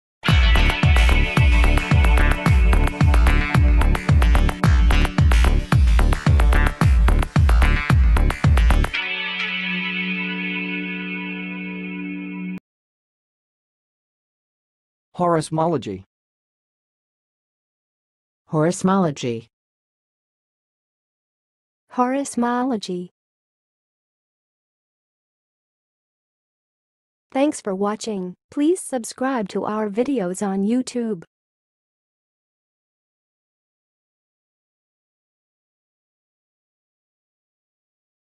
How to say "horismology"! (High Quality Voices)
Watch in this video how to say and pronounce "horismology"!